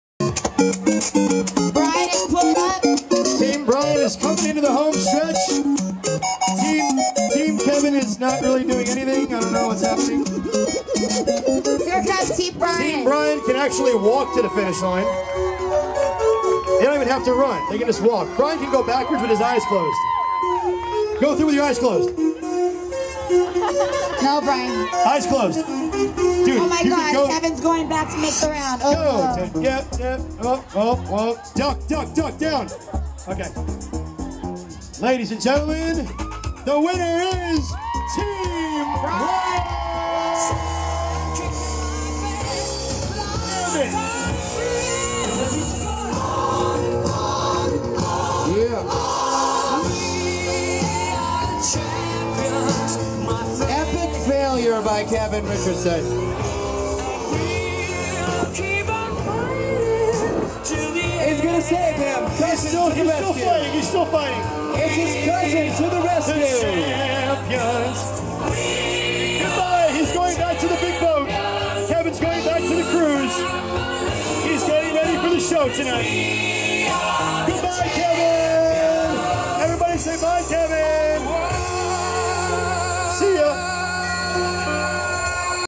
BSB Cruise 2013- Beach Party: Obstacle Course Brian vs Kevin
Check out my blog for more info on Cruising with the Backstreet Boys bsbcruise.blogspot.com